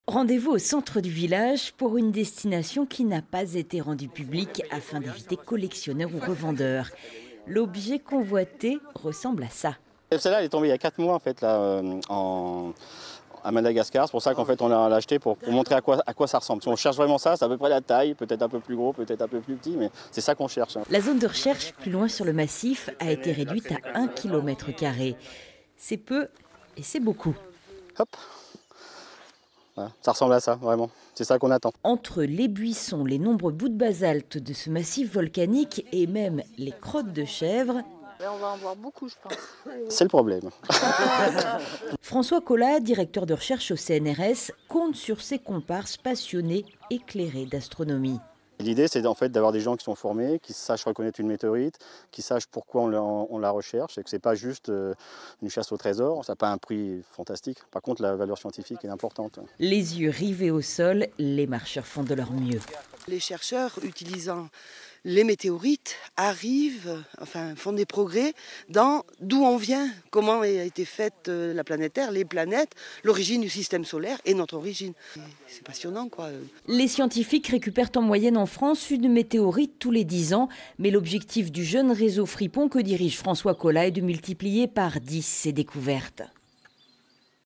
Ardèche : à la recherche de la météorite de Sceautres
Une météorite est tombé sur le massif du Coiron, en Ardèche, le mercredi 27 février. Une première campagne de fouilles a eu lieu le dimanche 3 mars sans rien donner. Les recherches ont repris une semaine plus tard sur la commune de Sceautres.
Reportage : Vanessa Fize et Nicolas Ferro

Retrouvez-nous sur :
Notre site web